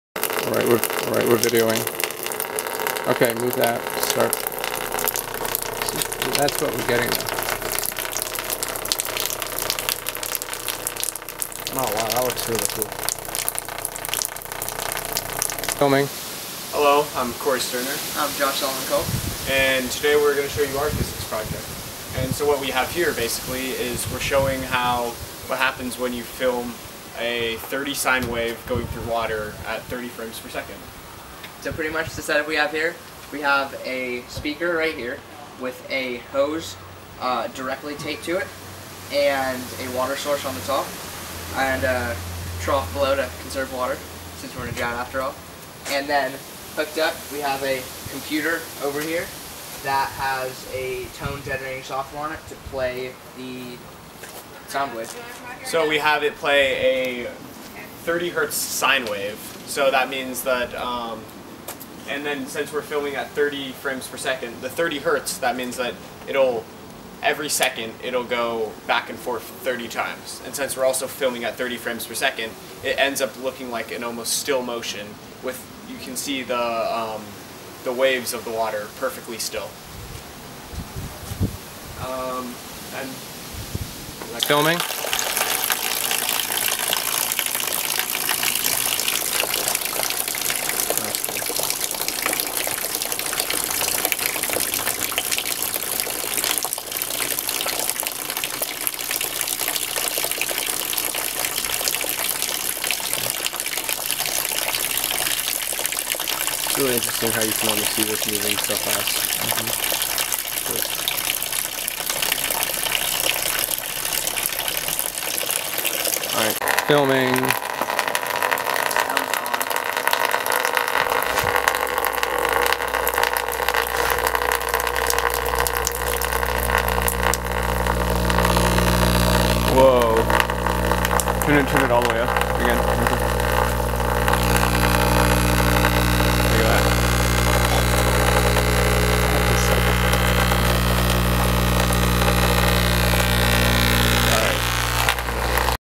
30Hz wave through water physics project
My friend and I decided to make this for our physics project and after several setbacks where we thought we couldn't make it work we finally were able to get the entire set up working. There is another more popular video showing the same process at 24Hz but the instructions were unclear and we could't get it working for a while

What You need:
A sub-woofer or speaker capable of going to 24Hz or 30Hz
Simple tone generating software (You can find it online for a computer or in an app store)
Something to mount it on
A flexible hose (preferably vinyl)

You can feed water through the hose using pressure but we found it was better to just siphon it.